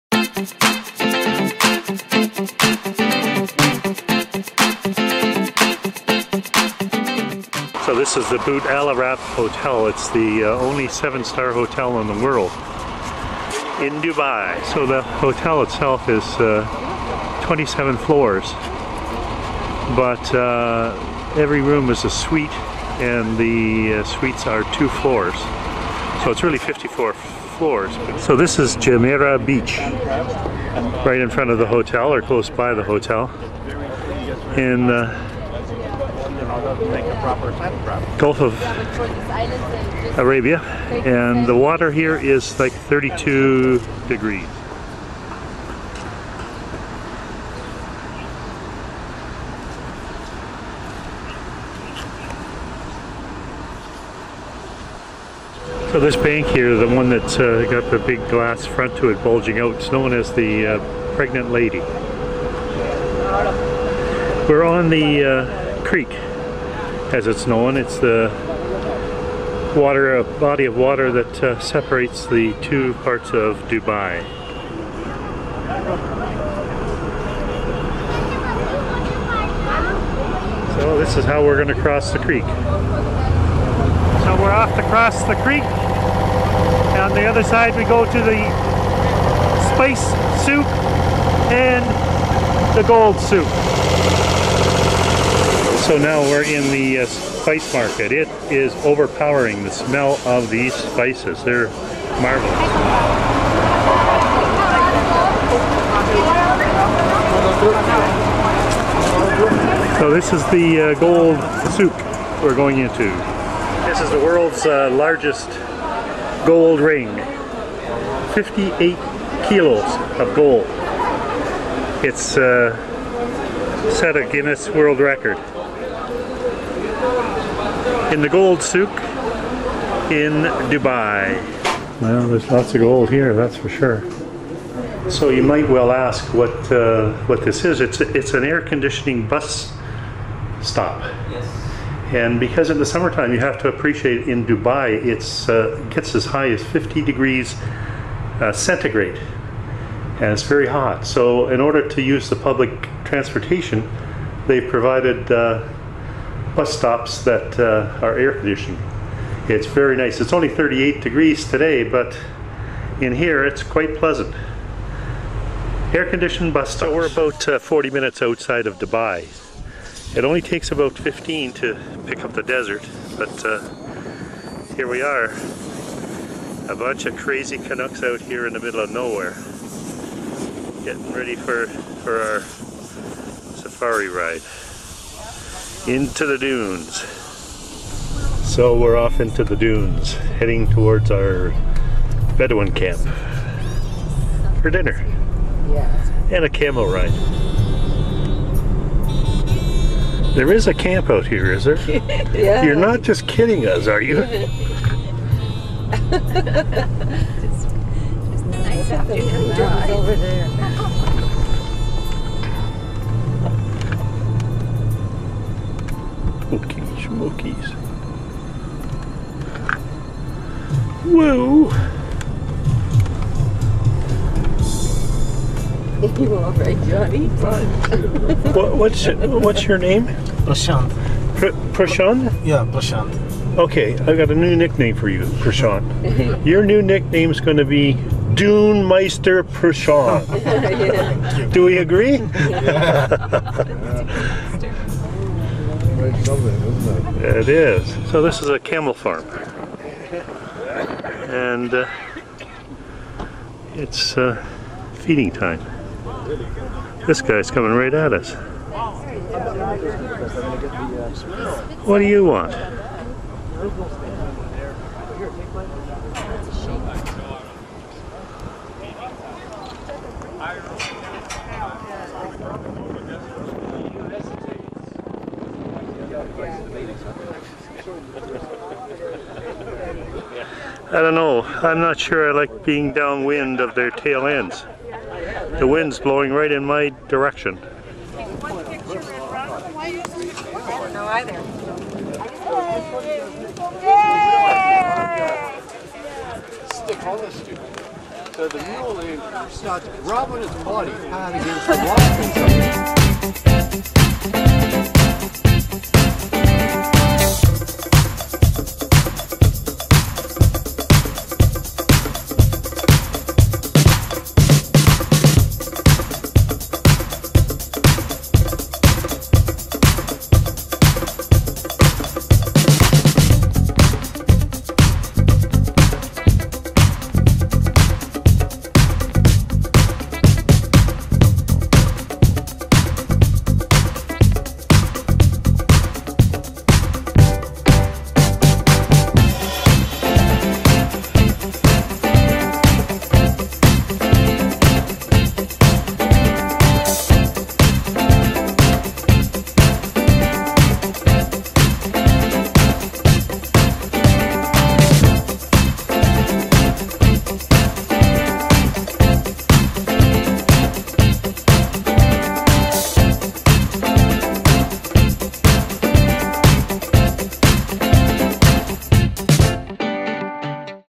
Dubai in the UNITED ARAB EMIRATES
The dunes, gold, spices, spectacular architecture and of course sand. this is magical, exotic Dubai.

The picture of the veiled lady was one of the women in our tour group dressed in local attire.